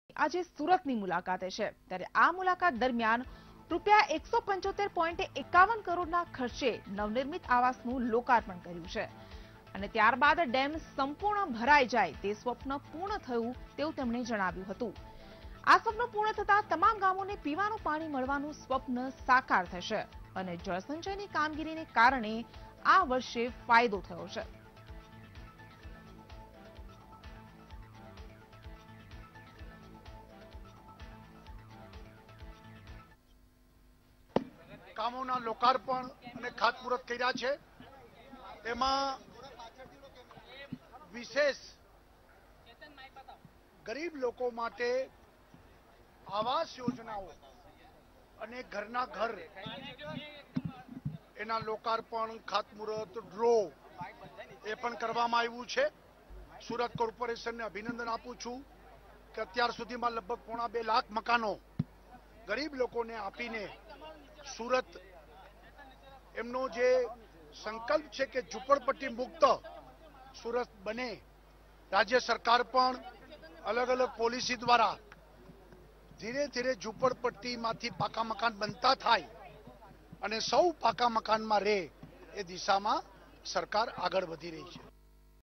CM Vijay Rupani Surat શહેરની મુલાકાતે, EWS-II ના 3 હજાર 951 આવસોની ફાળવણી | VTV Gujarati News
CM Vijay Rupani Surat શહેરની મુલાકાતે, EWS-II ના 3 હજાર 951 આવસોની ફાળવણી